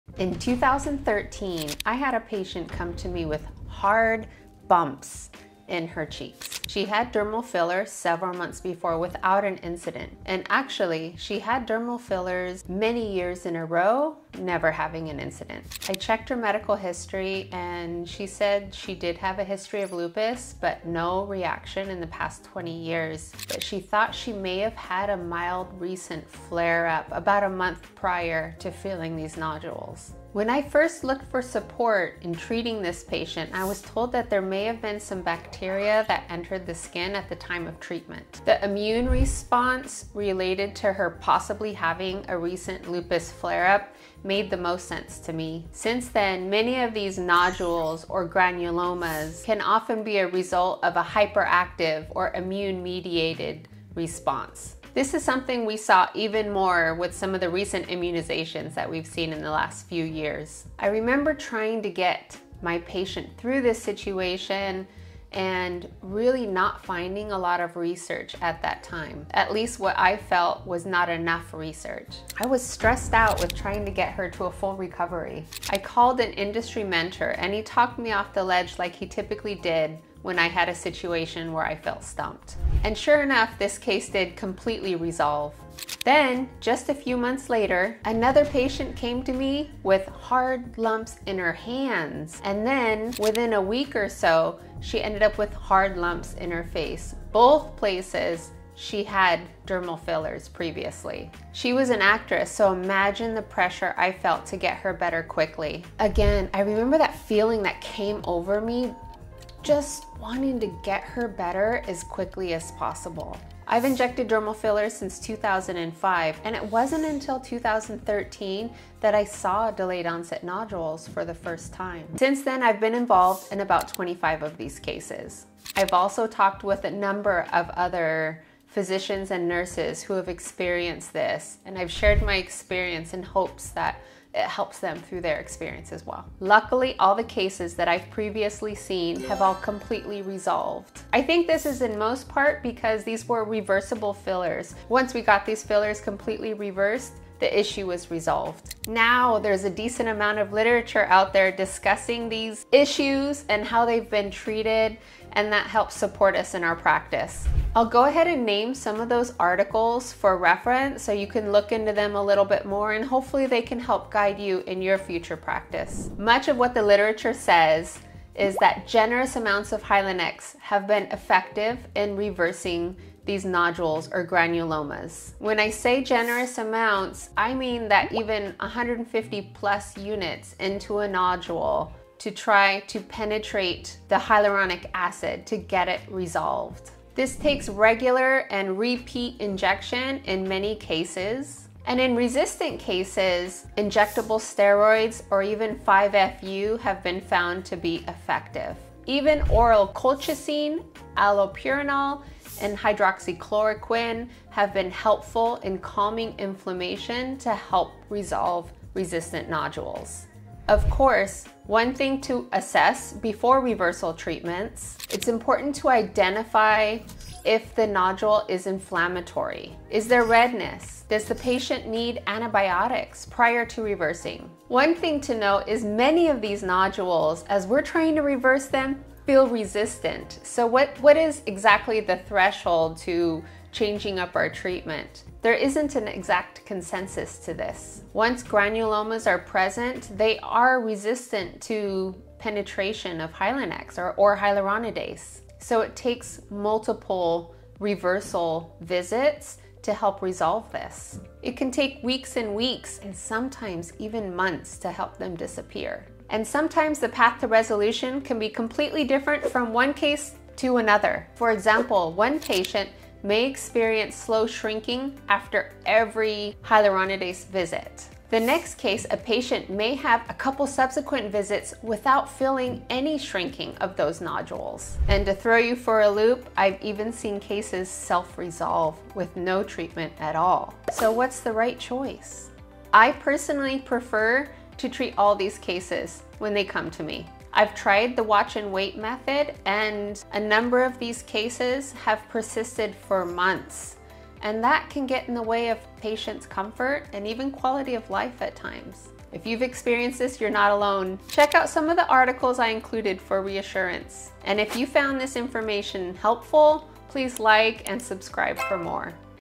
Delayed Onset Nodules after Dermal Fillers: Treatment Options
Years ago the research on how to treat delayed onset nodules after dermal fillers was very limited. We now have multiple sources of reliable information to help guide us in getting our patients to complete recovery. Here I have summarized what has been effective in my practice.

Convery C, Davies E, Murray G, Walker L. Delayed-onset Nodules (DONs) and Considering their Treatment following use of Hyaluronic Acid (HA) Fillers. J Clin Aesthet Dermatol. 2021 Jul;14(7):E59-E67. Epub 2021 Jul 1. PMID: 34840652; PMCID: PMC8570356.

Funt DK. Treatment of Delayed-onset Inflammatory Reactions to Hyaluronic Acid Filler: An Algorithmic Approach. Plast Reconstr Surg Glob Open. 2022 Jun 20;10(6):e4362. doi: 10.1097/GOX.0000000000004362. PMID: 35747256; PMCID: PMC9208893.

Lee W, Shah-Desai S, Rho NK, Cho J. Etiology of Delayed Inflammatory Reaction Induced by Hyaluronic Acid Filler. Arch Plast Surg. 2024 Feb 7;51(1):20-26. doi: 10.1055/a-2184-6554. PMID: 38425859; PMCID: PMC10901605.